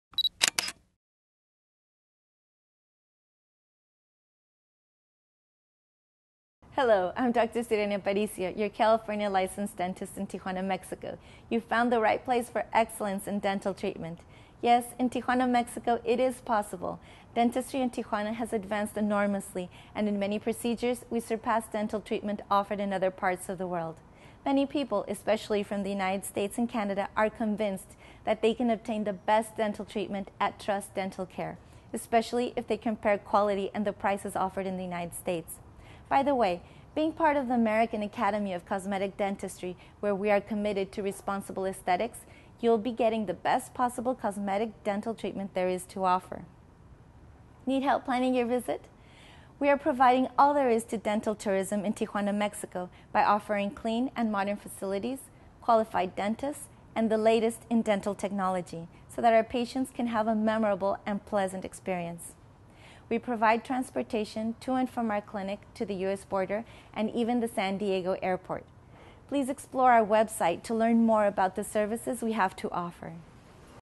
Green Screen & Audio (lavalier test)
This is a sample from a dental commercial I shot using a green screen and a lavalier microphone. The idea of this shot is to utilize custom graphics in the background or put my subject in any environment. In addition, I wanted to test the functions of a lavalier microphone.